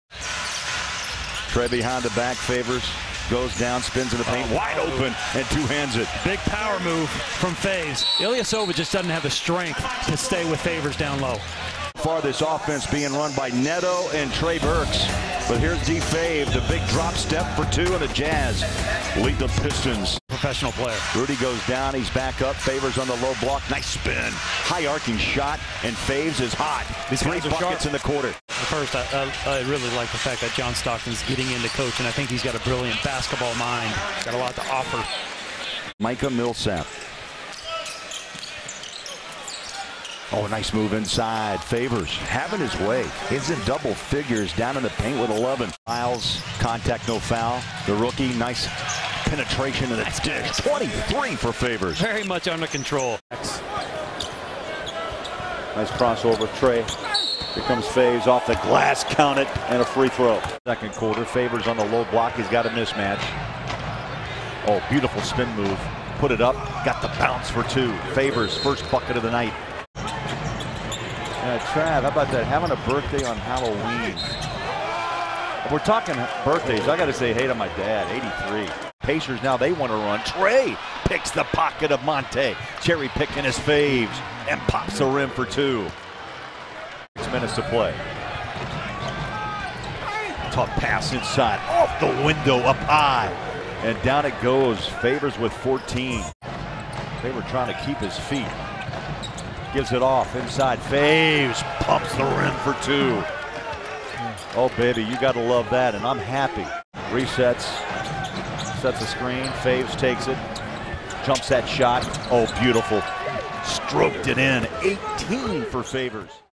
Derrick Favors vs. DET/PHI/IND
Through 3 games, Derrick Favors is averaging 21.3 points, 8.3 rebounds, 2.3 steals and 1.3 blocks per game.